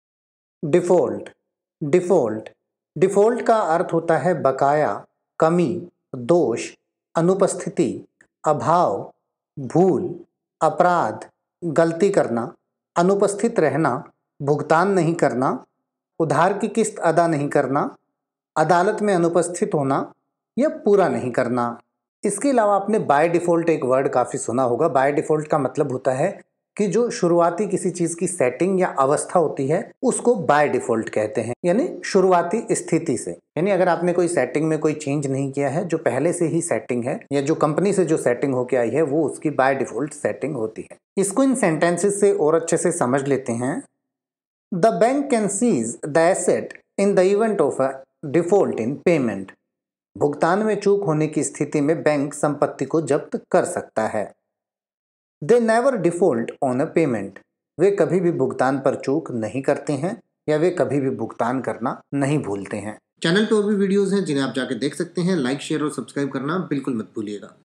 Default meaning in Hindi | Default का हिंदी में अर्थ | explained Default in Hindi
इस वीडियो में आप Default का हिंदी में मतलब समझेंगे और साथ में Default का pronunciation भी सीखेंगें यानि की आप Default के अर्थ के साथ साथ ये भी सीखेंगें की Default को कैसे बोले या Default को बोलने का सही तरीका क्या है |

तो बस एक मिनट में सीखिए Default को|
Let’s learn Hindi Meaning of Default in detail.

Default का हिंदी में अर्थ होता है

Default को इन उदाहरणों से और अच्छे से समझ सकते हैं
The bank can seize the asset in the event of a default in payment.

भुगतान में चूक होने की स्थिति में बैंक संपत्ति को जब्त कर सकता है।

They never default on a payment.

वे कभी भी भुगतान पर चूक नहीं करते हैं।

For more such videos on spoken English and online English speaking course